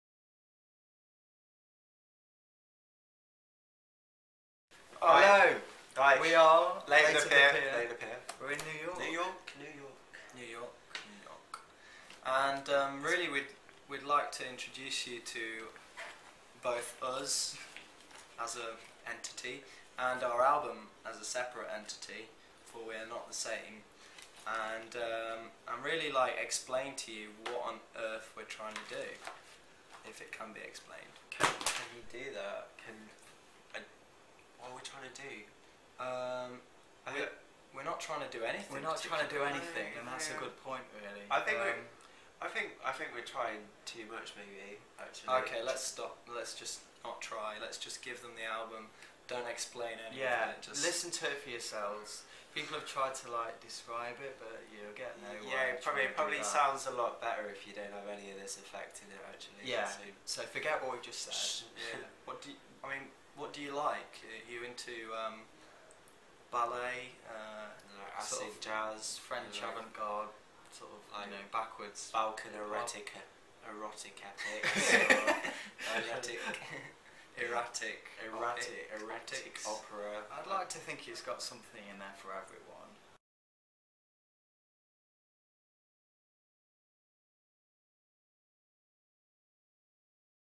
Late of the Pier Introduce Themselves
Big Shot met three of the four lads who comprise Late of the Pier a few hours before their first US show at Music Hall of Williamsburg.